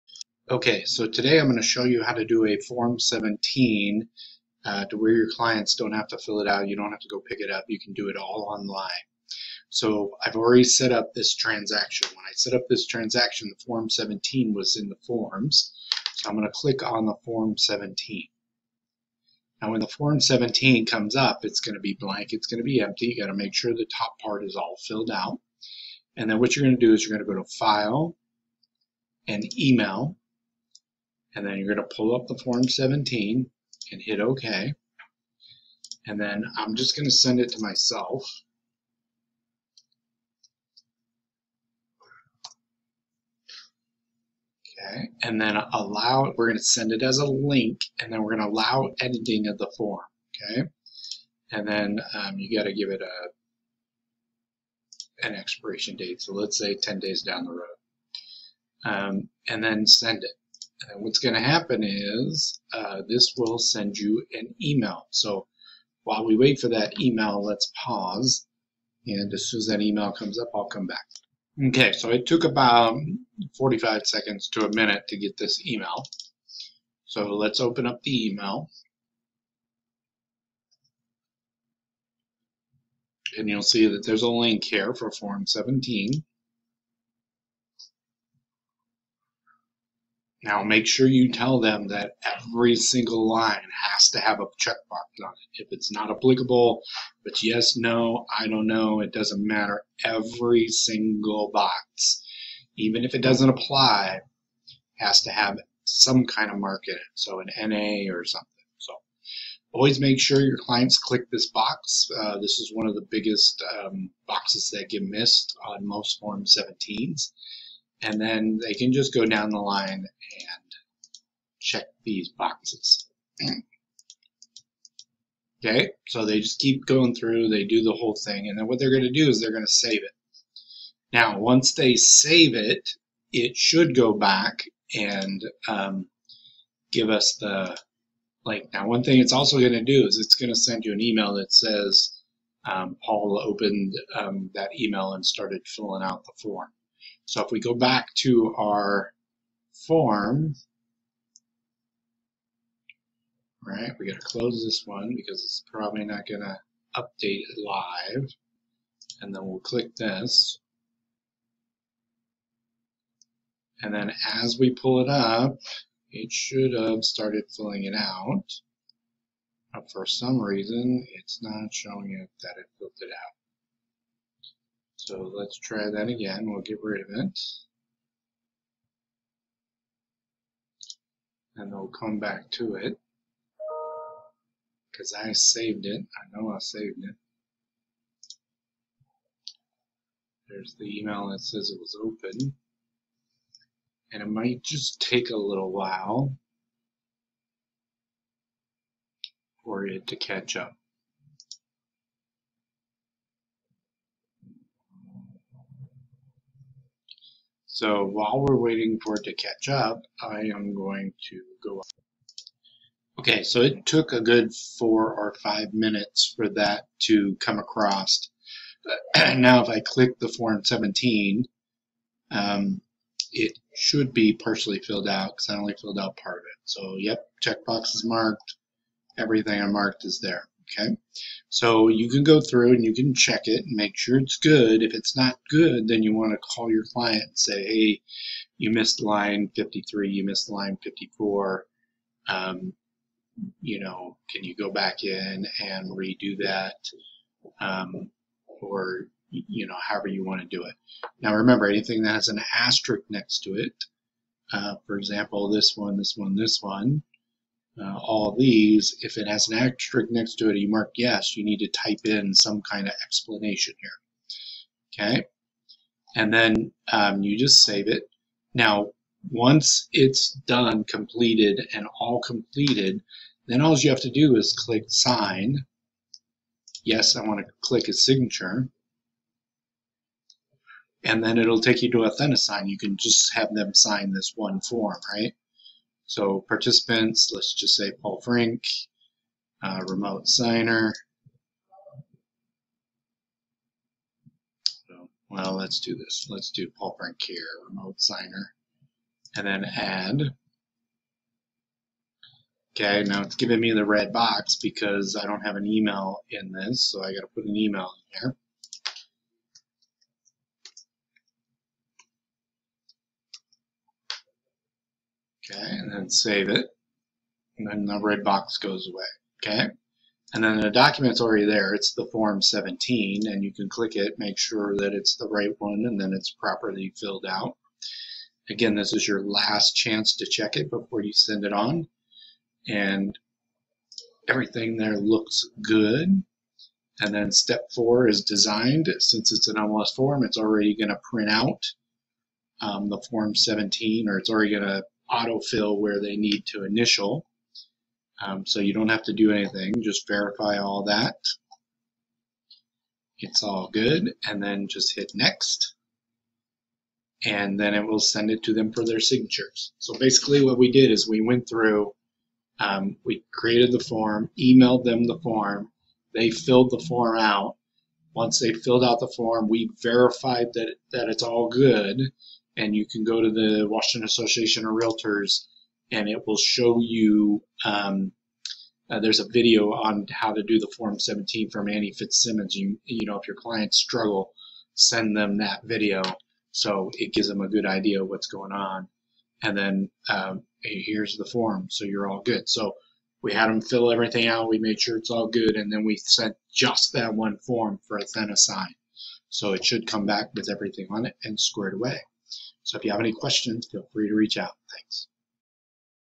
How to get the Form 17 filled out and signed via Transaction Desk
This video show how to get the Form 17 Seller's Disclosure form completely filled out and signed via Authentisign and Transaction Desk.